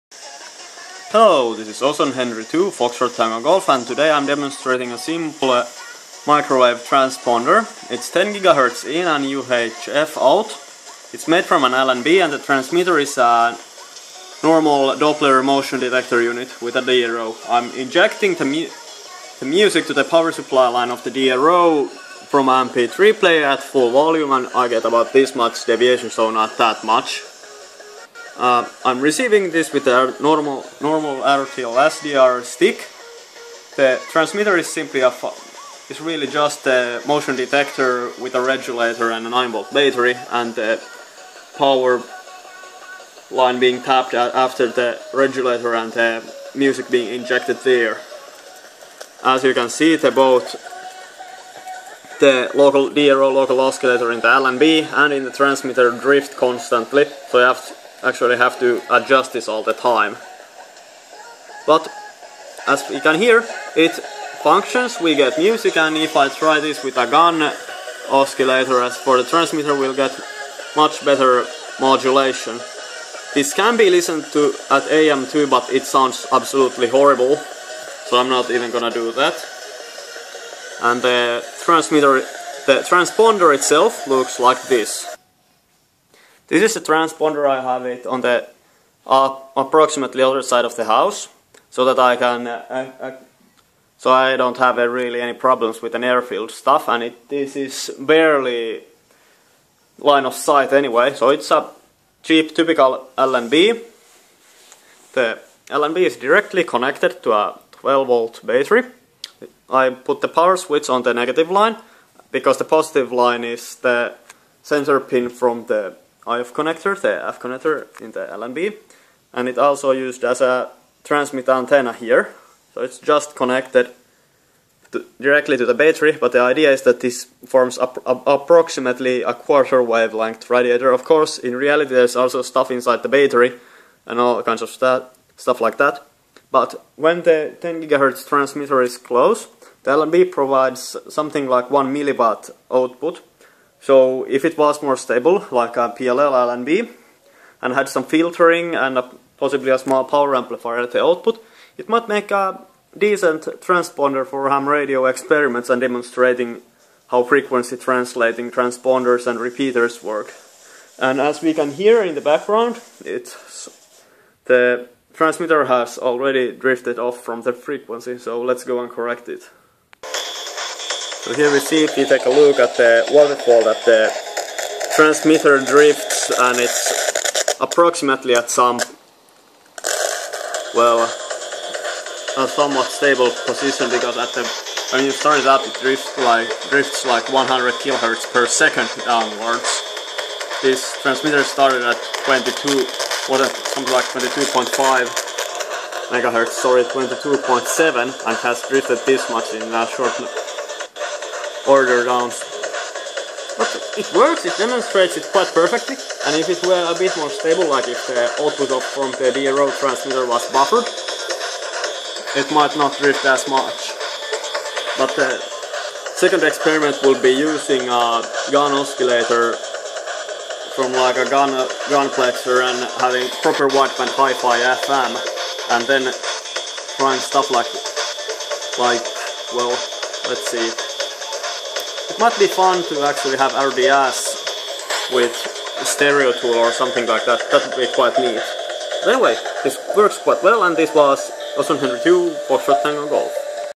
Simple microwave transponder experiment
Yet another shaky production from the lab of OH2FTG. 10.5GHz in UHF out.